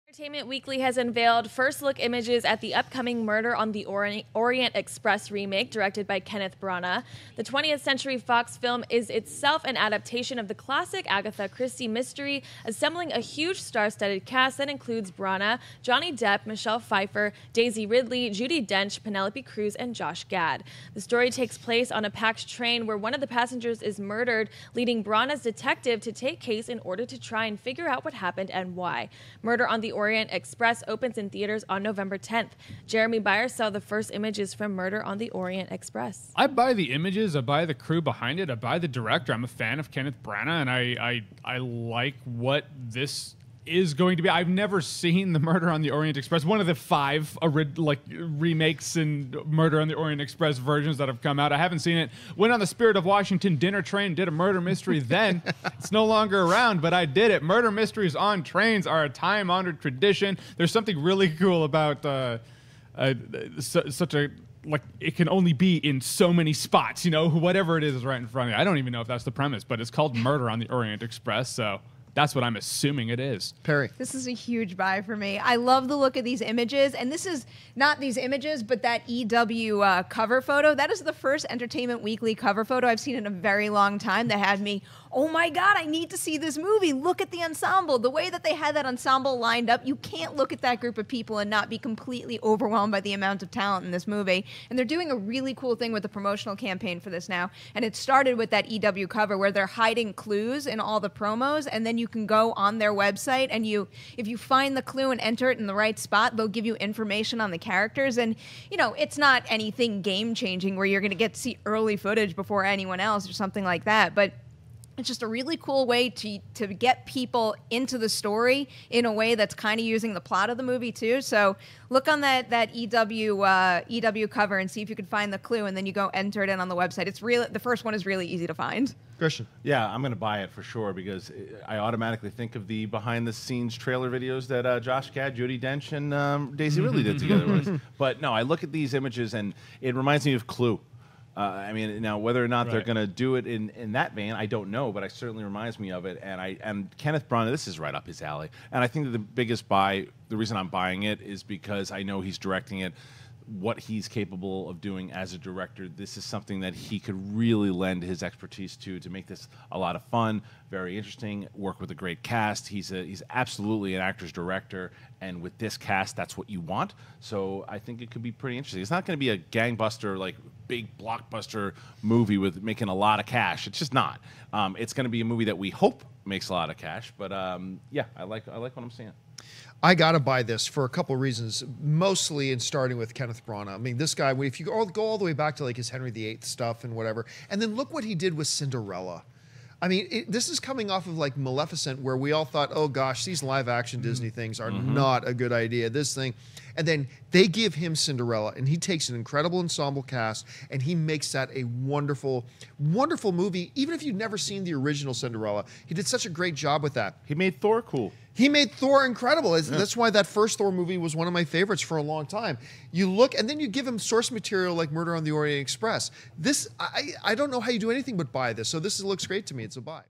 First Look at Kenneth Branagh's 'Murder on the Orient Express' Remake - Collider Video
Entertainment Weekly has unveiled first look images at the upcoming Murder on the Orient Express remake directed by Kenneth Branagh. The 20th Century Fox film is itself an adaptation of the classic Agatha Christie mystery, assembling a huge, star-studded cast that includes Branagh, Johnny Depp, Michelle Pfeiffer, Daisy Ridley, Judi Dench, Penelope Cruz and Josh Gad. See video for details.